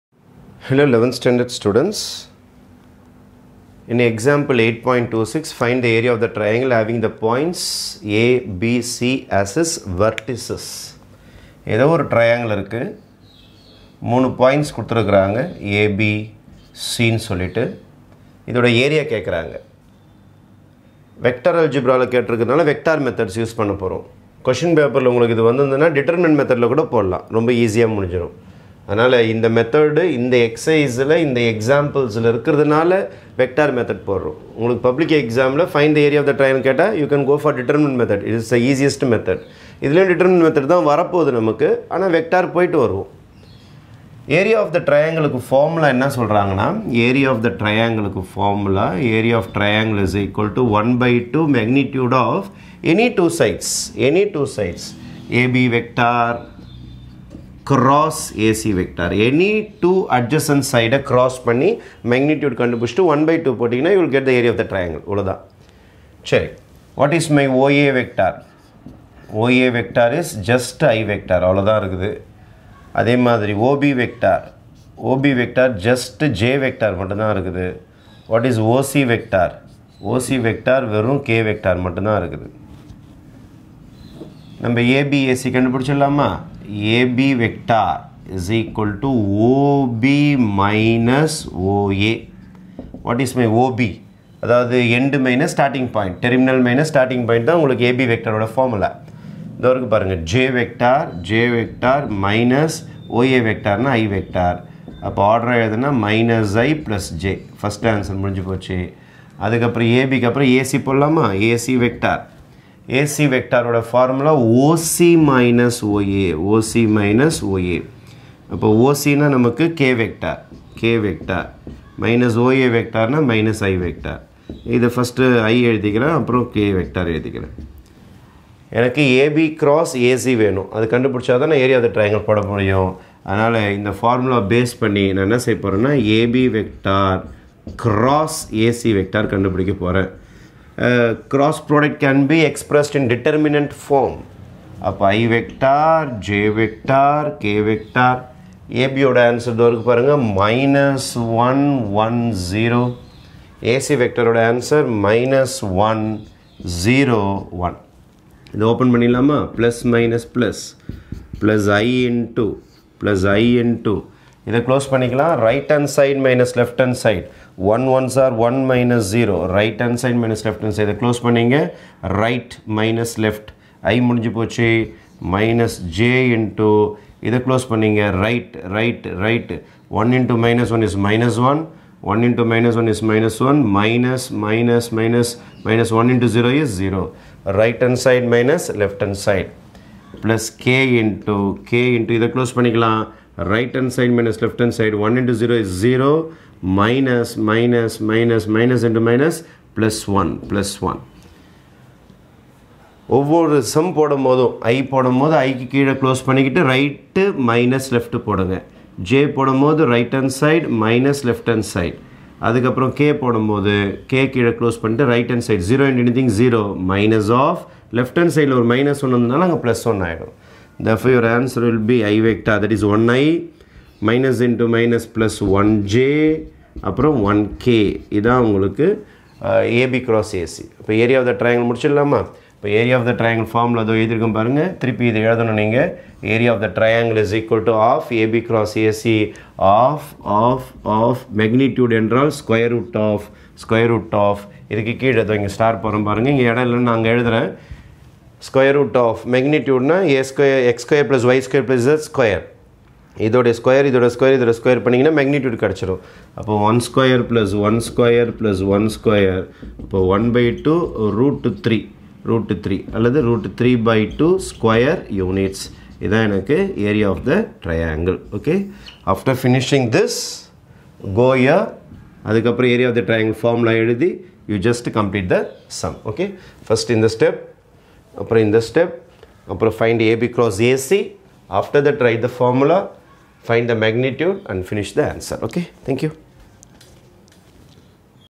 11th Std Maths Example 8.26 Find the area of the triangle A(1,0,0), B(0,1,0l and C(0,0,1) as it's ve
11th STD MATHS TAMILNADU STATEBOARD SYLLABUS

EXAMPLES